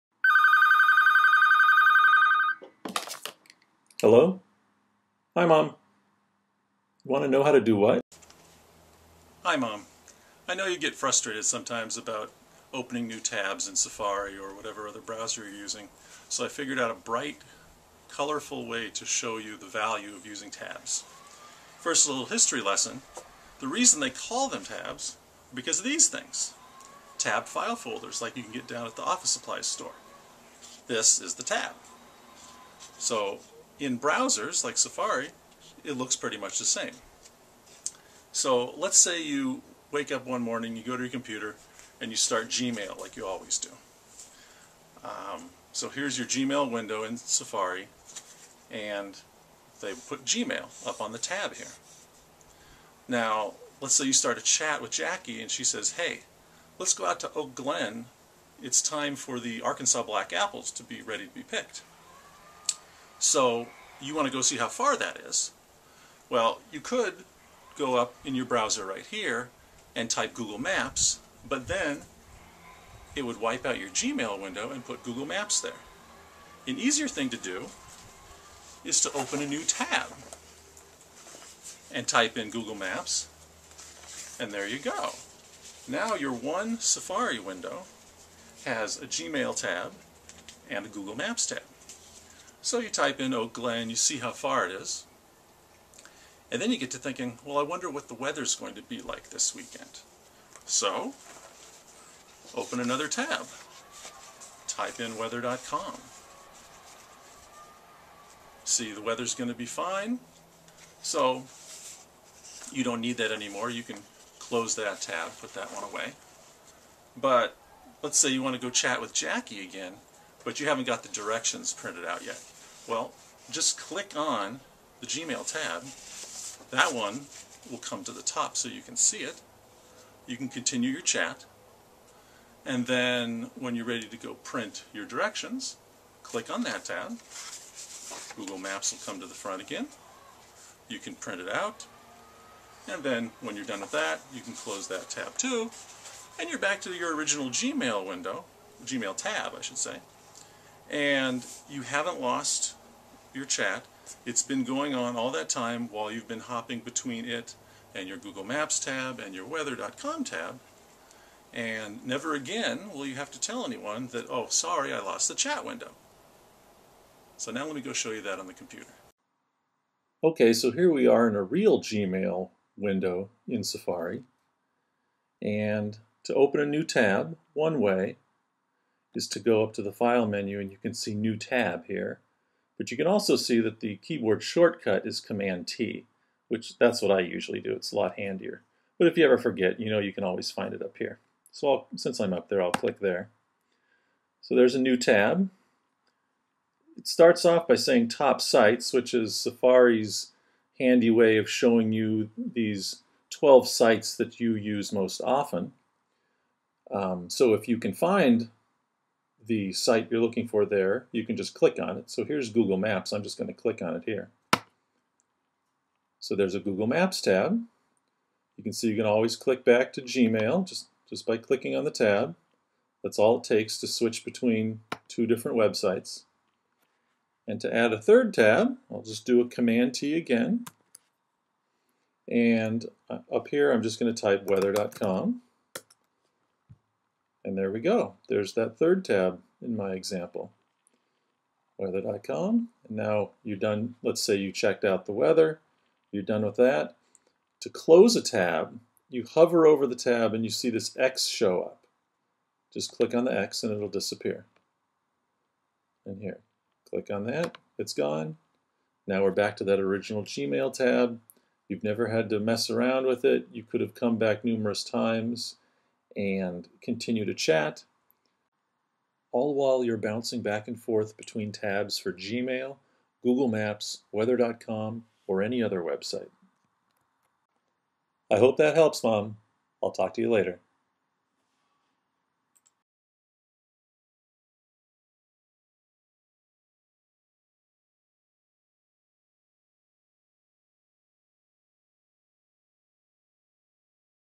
How do I use web browser tabs?
(Full article Hi Mom. I know you are sometimes frustrated by losing a chat window when surfing to another website so in this video I show you how using web browser tabs (like in Safari) can really make your life much easier.